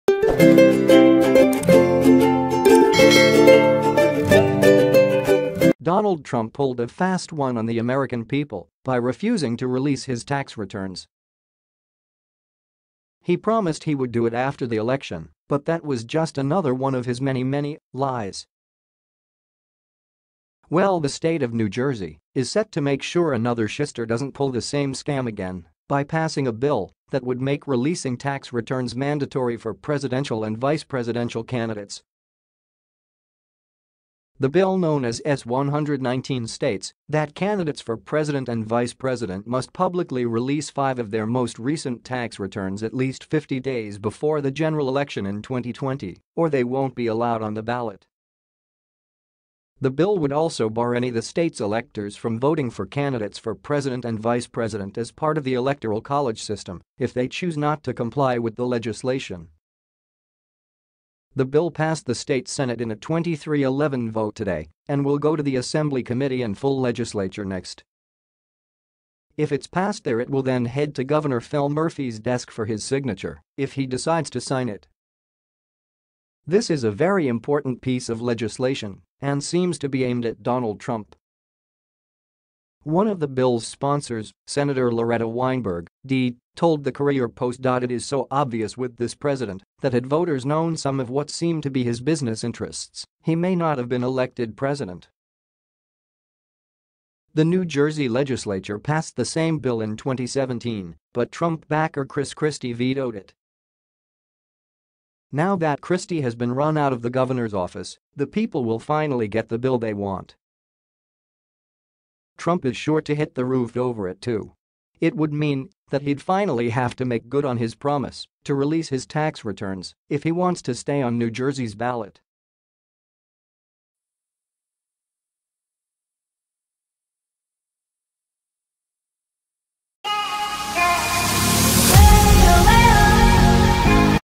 Senate Passes Bill That Would Keep Trump Off 2020 Ballot & He’s Pissed
Donald Trump pulled a fast one on the American people by refusing to release his tax returns.  He promised he would do it after the election, but that was just another one of his many, many, lies.  We